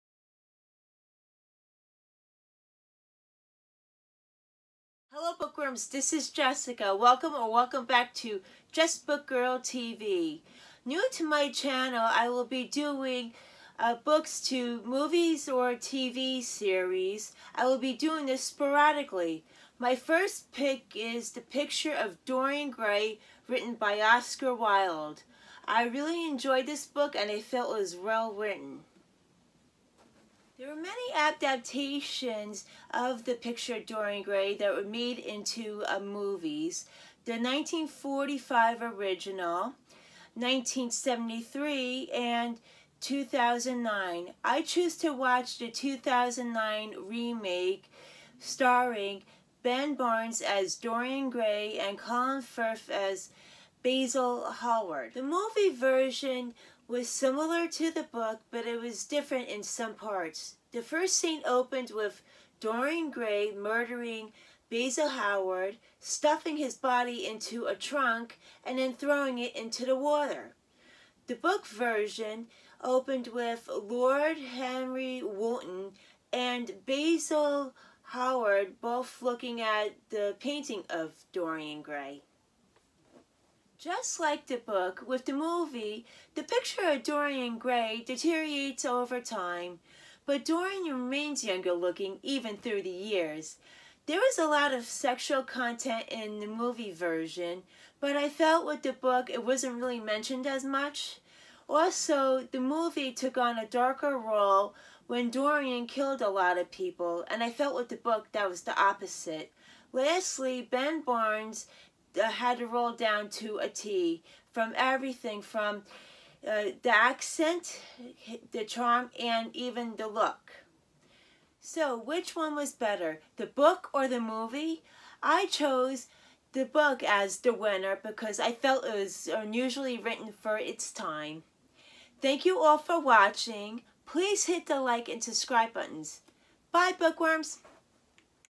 The Picture Of Dorian Gray Book Vs. The Movie
Recently, I read 'The Picture Of Dorian Gray,' by Oscar Wilde.  I also, watched the 2009 remake with Ben Barnes and Colin Firth.

Things Mentioned:
The Picture Of Dorian Gray by Oscar Wilde